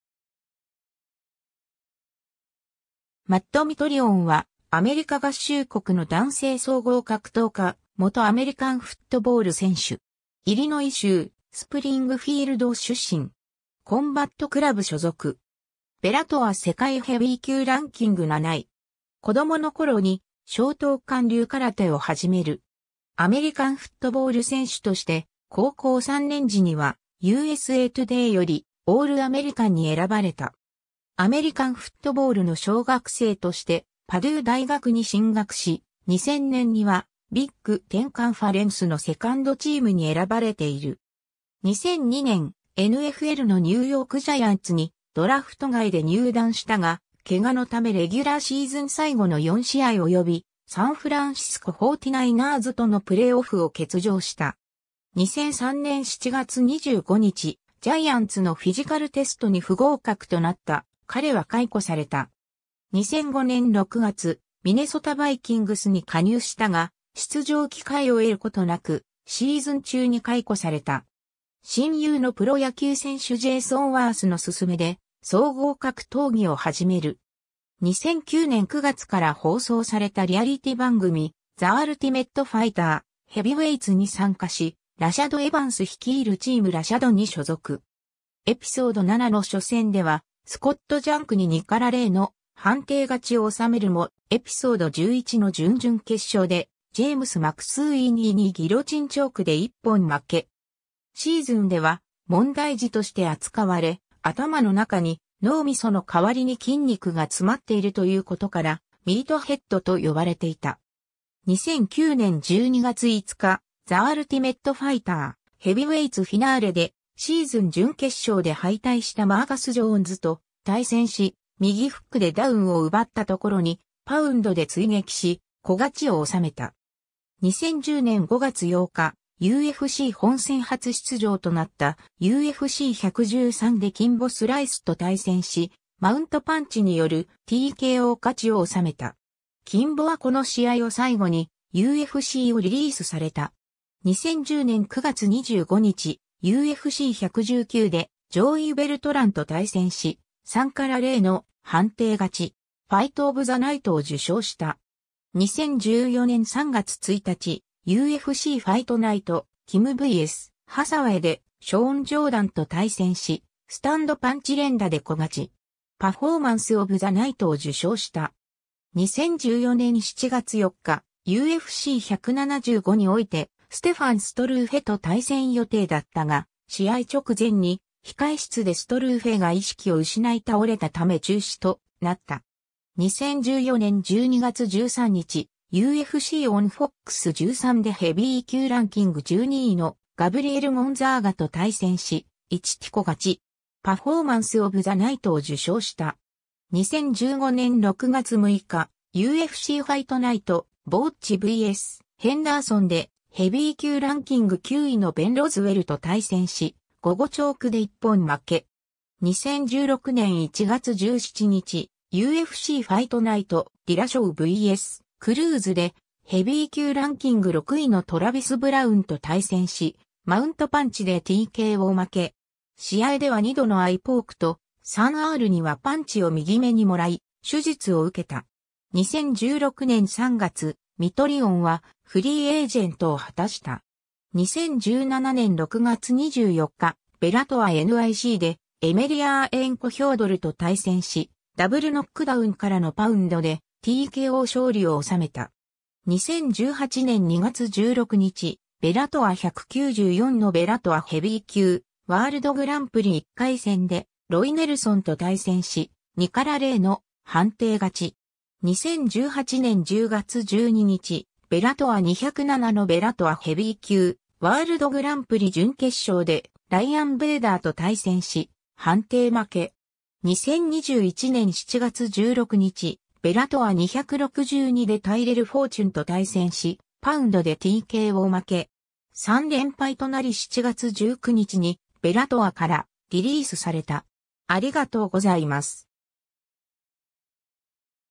マット・ミトリオン
ニューヨーク・ジャイアンツの選手
ミネソタ・バイキングスの選手
マット・ミトリオン（Matt Mitrione、1978年7月15日 - ）は、アメリカ合衆国の男性総合格闘家、元アメリカンフットボール選手。
イリノイ州スプリングフィールド出身。
コンバット・クラブ所属。
Bellator世界ヘビー級ランキング7位。
 子供の頃に松濤館流空手を始める。
アメリカンフットボール選手として、高校3年時にはUSAトゥデイよりオールアメリカンに選ばれた。
アメリカンフットボールの奨学生としてパデュー大学に進学し、2000年には、ビッグ・テン・カンファレンスのセカンドチームに選ばれている。
2002年、NFLのニューヨーク・ジャイアンツにドラフト外で入団したが、怪我のためレギュラーシーズン最後の4試合及びサンフランシスコ・フォーティナイナーズとのプレーオフを欠場した。
2003年7月25日、ジャイアンツのフィジカルテストに不合格となった彼は解雇された。
2005年6月、ミネソタ・バイキングスに加入したが、出場機会を得ることなく、シーズン中に解雇された。
 親友のプロ野球選手ジェイソン・ワースの勧めで総合格闘技を始める。
 2009年9月から放送されたリアリティ番組「The Ultimate Fighter: Heavyweights」に参加し、ラシャド・エヴァンス率いるチーム・ラシャドに所属。
エピソード7の初戦ではスコット・ジャンクに2-0の判定勝ちを収めるも、エピソード11の準々決勝でジェームス・マクスウィーニーにギロチンチョークで一本負け。
シーズンでは問題児として扱われ、頭の中に脳ミソの代わりに筋肉が詰まっているということから「ミートヘッド」（日本語吹き替え版では「アホちん」）と呼ばれていた。
 2009年12月5日、The Ultimate Fighter: Heavyweights Finaleでシーズン準決勝で敗退したマーカス・ジョーンズと対戦し、右フックでダウンを奪ったところにパウンドで追撃しKO勝ちを収めた。
 2010年5月8日、UFC本戦初出場となったUFC 113でキンボ・スライスと対戦し、マウントパンチによるTKO勝ちを収めた。
キンボはこの試合を最後にUFCをリリースされた。
 2010年9月25日、UFC 119でジョーイ・ベルトランと対戦し、3-0の判定勝ち。
 2014年3月1日、UFC Fight Night: Kim vs. Hathawayでショーン・ジョーダンと対戦し、スタンドパンチ連打でKO勝ち。
 2014年7月4日、UFC 175においてステファン・ストルーフェと対戦予定だったが、試合直前に控え室でストルーフェが意識を失い倒れたため中止となった。
 2014年12月13日、 UFC on FOX 13でヘビー級ランキング12位のガブリエル・ゴンザーガと対戦し、1RTKO勝ち。
 2015年6月6日、UFC Fight Night: Boetsch vs. Hendersonでヘビー級ランキング9位のベン・ロズウェルと対戦し、ゴゴチョークで一本負け。
 2016年1月17日、UFC Fight Night: Dillashaw vs. Cruzでヘビー級ランキング6位のトラヴィス・ブラウンと対戦し、マウントパンチでTKO負け。
試合では2度のアイポークと、3Rにはパンチを右目にもらい手術を受けた。
 2016年3月、ミトリオンはフリーエージェントを果たした。
 2017年6月24日、Bellator NYCでエメリヤーエンコ・ヒョードルと対戦し、ダブルノックダウンからのパウンドでTKO勝利を収めた。
 2018年2月16日、Bellator 194のBellatorヘビー級ワールドグランプリ1回戦でロイ・ネルソンと対戦し、2-0の判定勝ち。
 2018年10月12日、Bellator 207のBellatorヘビー級ワールドグランプリ準決勝でライアン・ベイダーと対戦し、判定負け。
 2021年7月16日、Bellator 262でタイレル・フォーチュンと対戦し、パウンドでTKO負け。